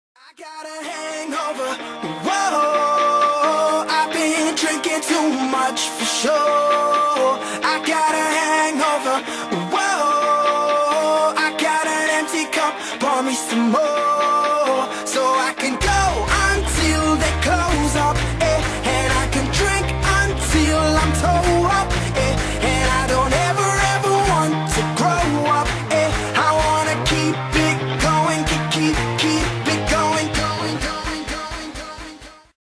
Mi primera animación =3
Bueno, la historia va sobre de que Neus llego a Mobius y ella pensaba que jamás la aceptarian en Mobius, y como la trataron mal en su planeta siempre pensó que también en Mobius la tratarían igual, pero no fue así y por eso se puso como en la animación =3 Saludos